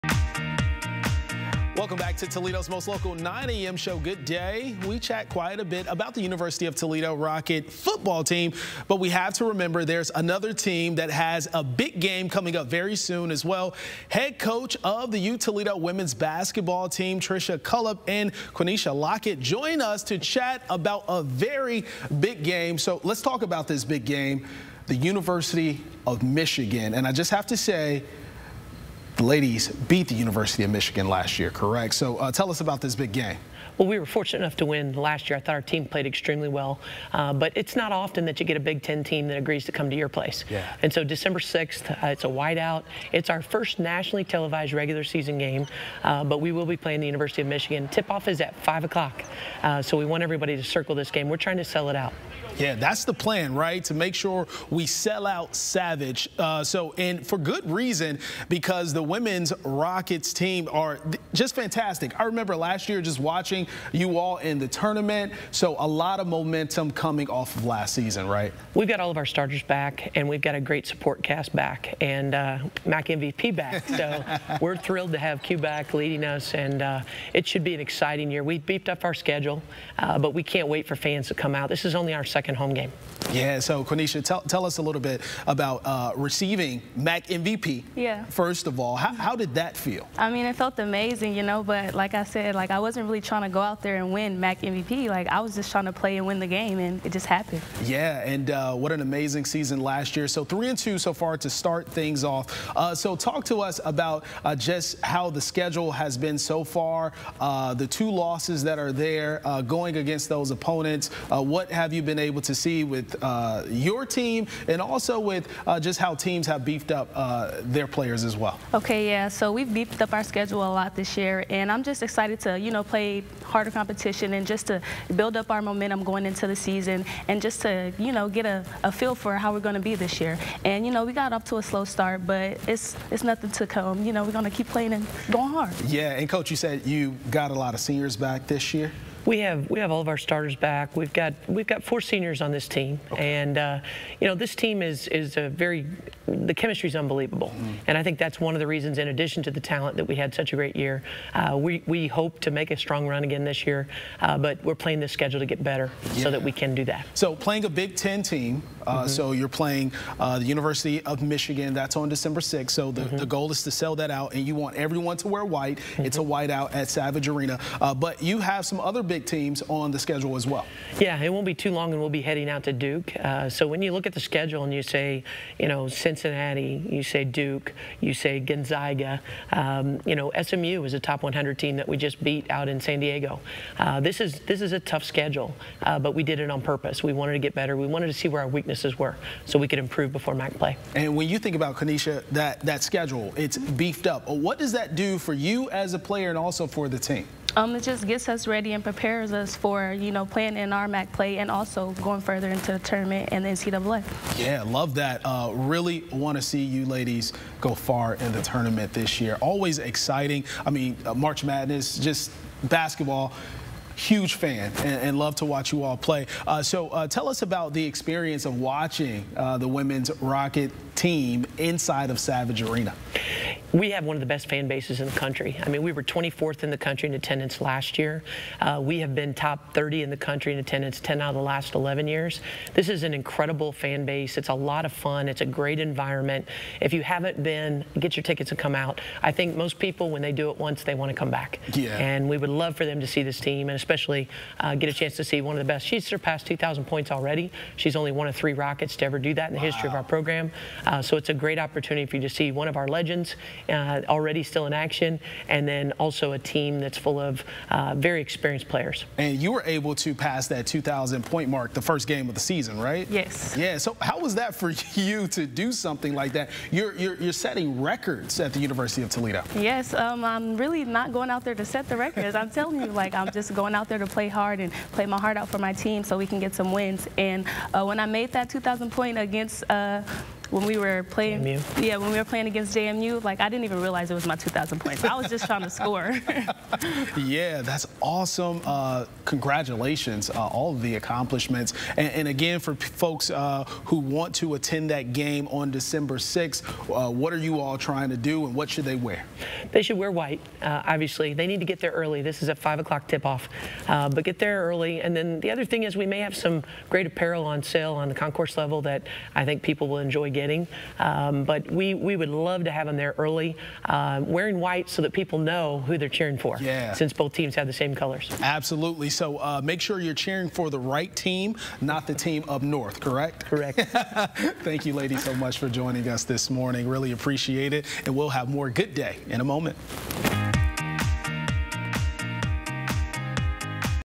Rockets women's basketball face off against Wolverines | Good Day on WTOL 11
Head coach of the UToledo women's basketball team Tricia Cullop and Quinesha Lockett talk the upcoming game against the University of Michigan.